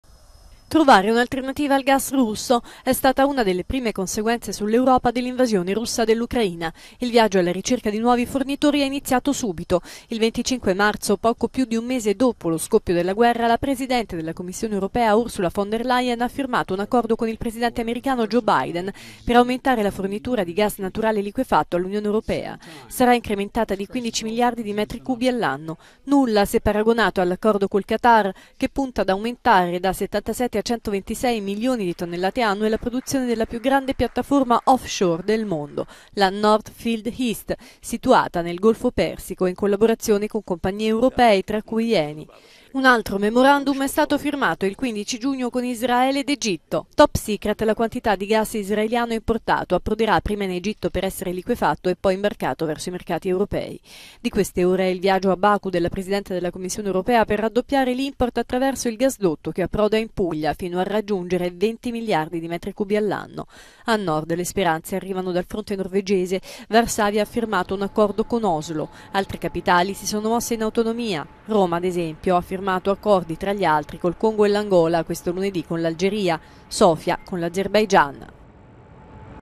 Da dove verrà il gas? Mappa delle nuove forniture per l'Europa
Mentre Mosca diminuisce il flusso di gas verso l'Europa, Bruxelles e le singole capitali si muovono per cercare fornitori alternativi, il punto